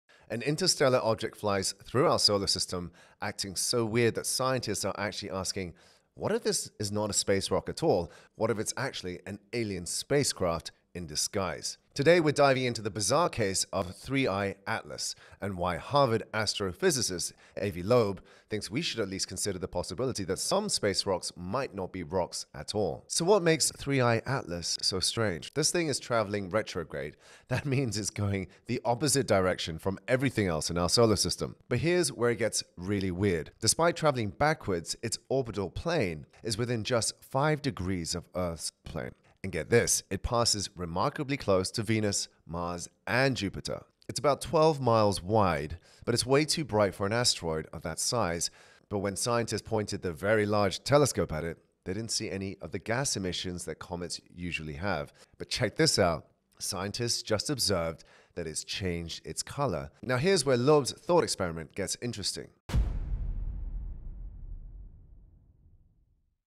🚨 This space rock is breaking the rules!
Harvard scientist Avi Loeb says 3IAtlas might not be a rock at all… but an extraterrestrial spacecraft in disguise. 👁️

It’s flying backwards.
It’s too bright.
And it’s coming back soon. ☄️